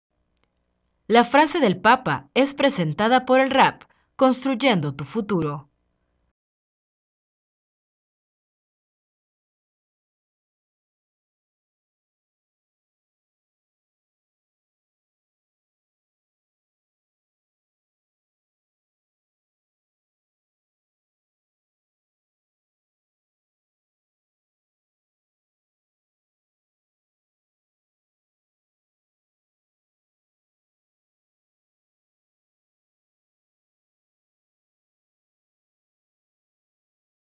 Frase del Papa
La cercanía al Señor invita a nuestro corazón a acercarse con amor a nuestros hermanos y hermanas, a tener una actitud de compasión hacia todos.